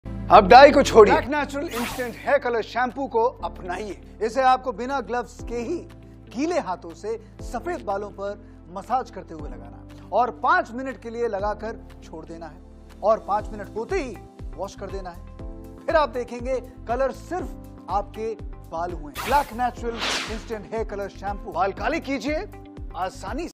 GREY HAIR SOLUTIONS IN 5 MINT HAND MAGIC | LUVVEL HAIR COLOR SHAMPOO | AMAZON PAKISTAN
CLICK LINK TO BUY LUVVEL HAIR DARKENING SHAMPOO, DARK BROWN, FOR MEN & WOMEN, 200ML
product Ki Tafseel Ky Liye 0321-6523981 Or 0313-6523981 Par Rabta krein
Hey Dosto,
Welcome to Amazon Pakistan,
Kiya Ap Pareshan hai Apny Saafid Balon ly Kr Iska Haal Kiya Hoga
Wo Hal Hai Luvvel Hair Color Shampoo Ab Apko Dosry Color Istemal Krny ki Koi Zaroorat Nh Ab
Shampoo Ki Tarah Laga Apny Safaid balon Ko Kala Krein.